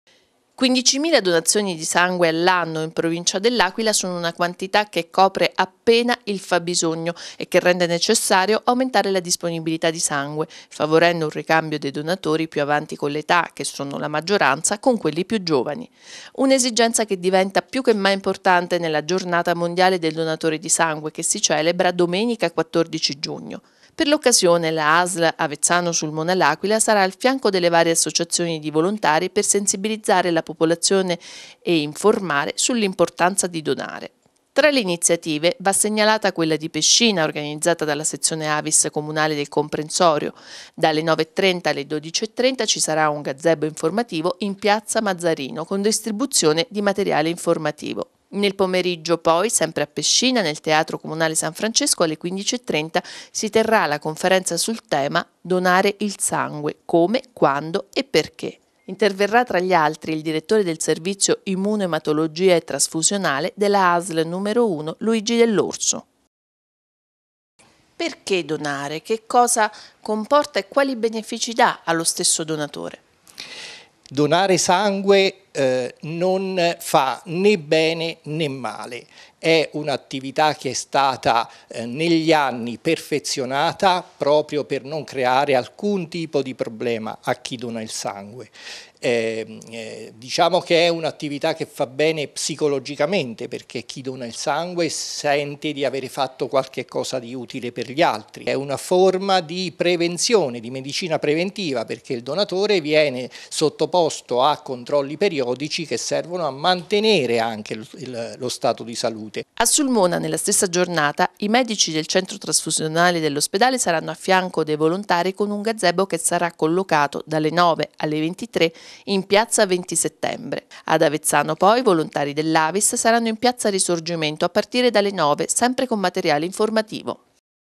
Giornata nazionale del donatore
Il sangue raccolto dalle donazioni basta solo per coprire i fabbisogni delle strutture ospedaliere. Serve sensibilizzare soprattutto per il ricambio generazionale. Grande il lavoro portato avanti dal centro trasfusionale del San Salvatore dell' Aquila